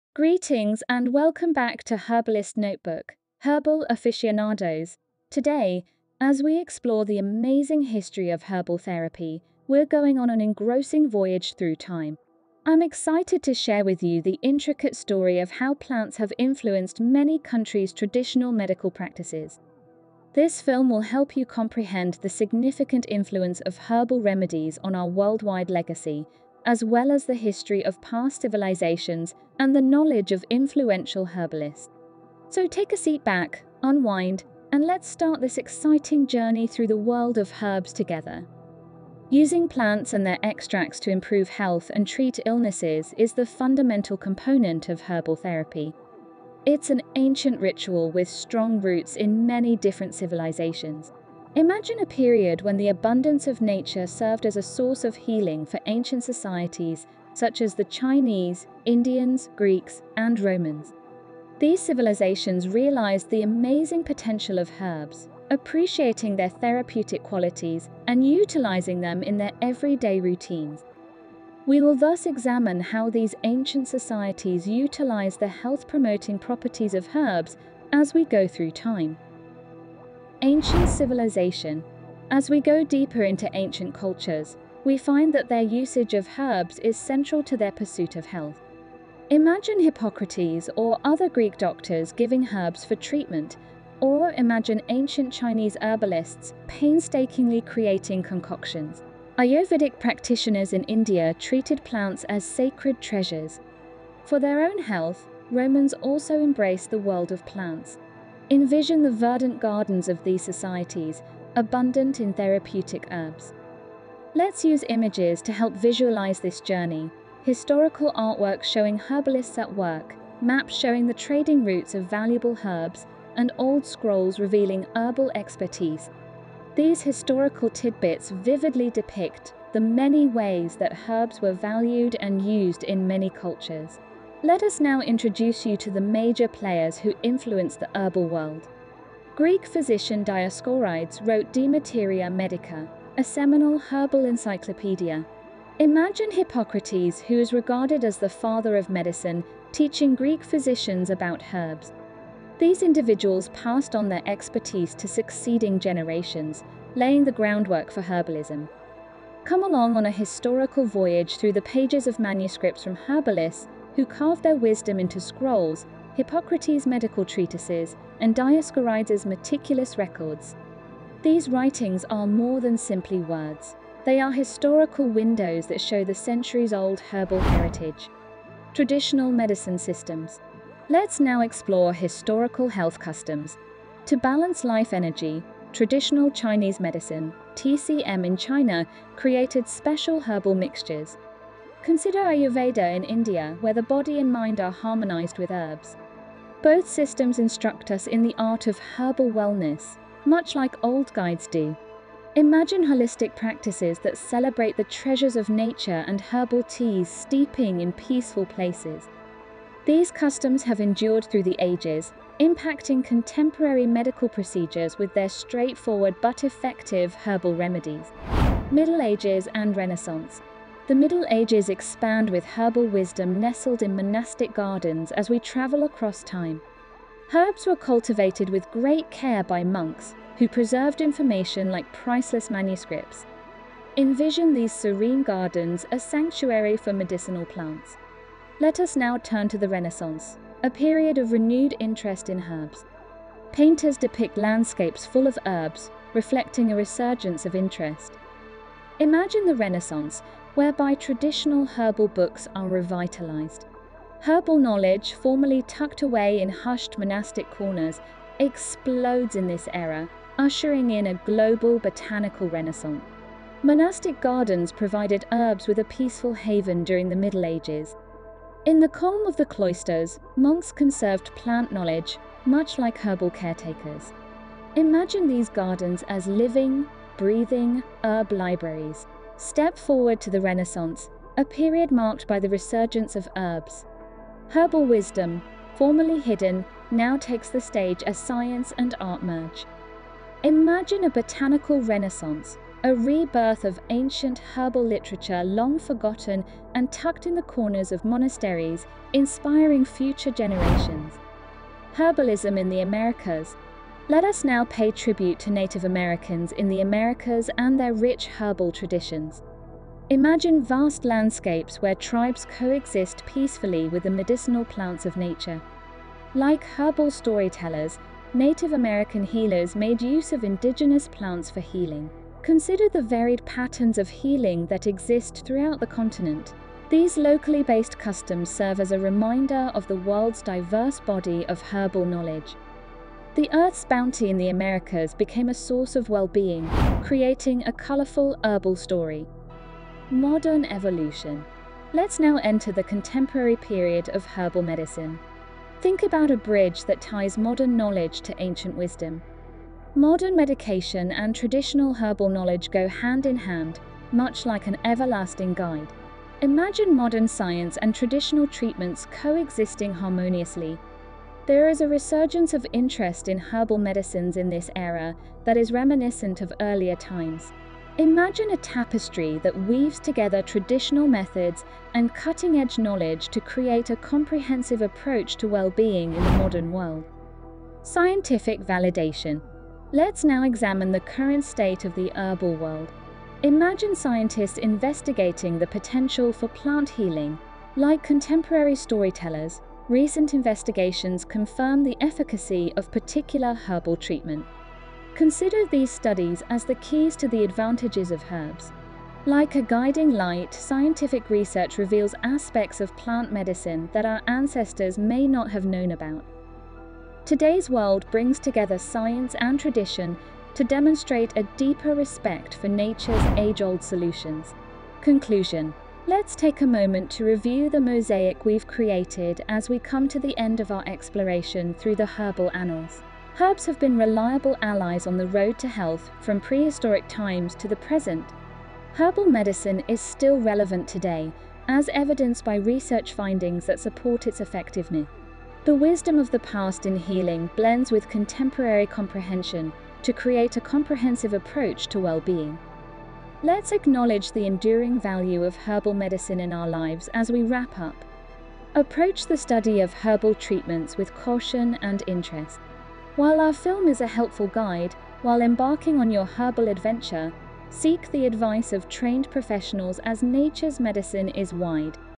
History of Herbal Medicine
In this insightful YouTube video, we embark on a fascinating journey through the "History of Herbal Medicine." Delving into the annals of time, we uncover the ancient roots and rich traditions that have shaped the world of herbal remedies. From the earliest civilizations to indigenous practices, the video explores how herbal medicine has played a pivotal role in human healthcare throughout history. Viewers will gain a deeper understanding of the diverse cultures and civilizations that have relied on the healing properties of plants, laying the foundation for the herbal knowledge we have today.

Join us as we unravel the secrets behind the botanical wisdom passed down through generations, examining the historical context of herbal medicine and its evolution into the modern era. Whether you're a wellness enthusiast, a history buff, or simply curious about alternative forms of healing, this video promises to be an enlightening exploration of the profound impact herbs have had on our health and well-being across centuries.